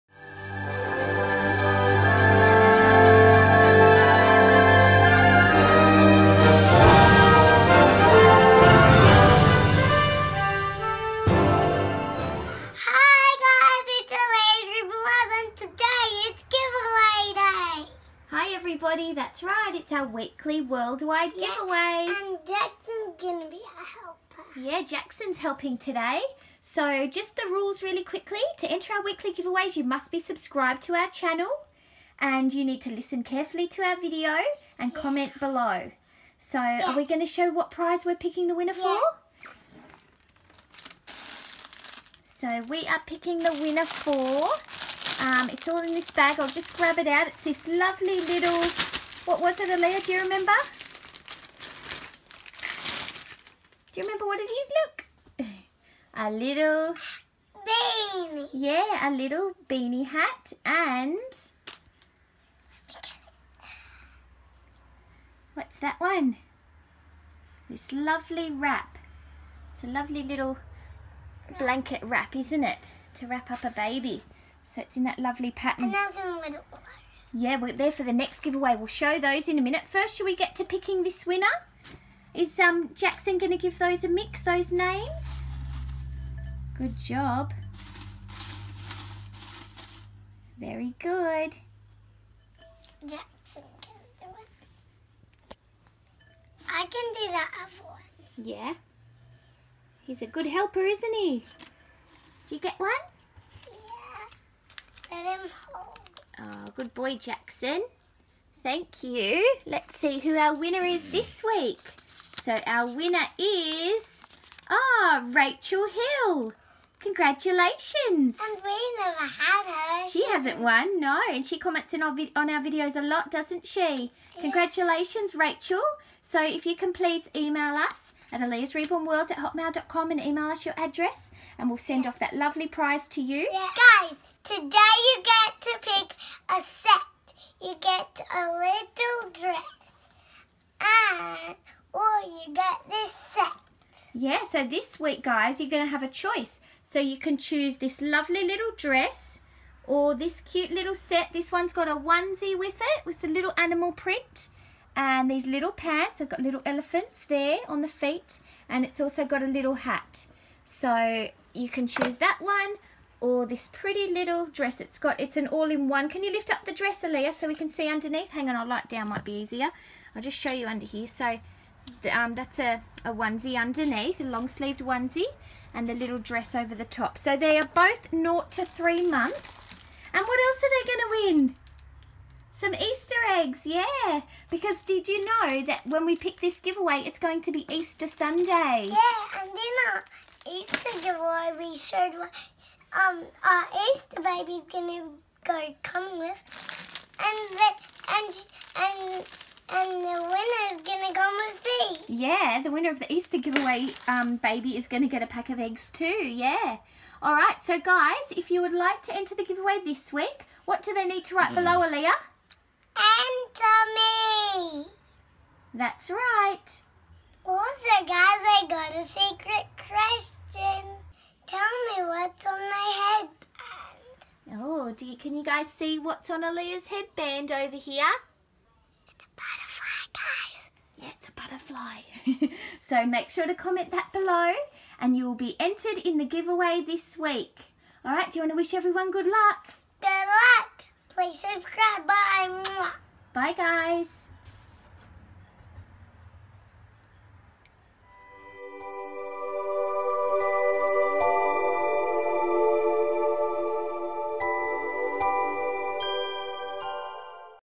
*ENTRIES NOW CLOSED* WEEKLY SUNDAY GIVEAWAY!! PLUS LAST WEEKS WINNER ANNOUNCED
Every week we give away a Reborn related item.
Make sure to watch the video each week to hear what you need to comment.
If you are under 18 please get permission from a parent or guardian before entering.

HAPPY MAIL WELCOME!
Please Post to:
Aliyah's Reborn World
Mulgoa NSW 2745
Australia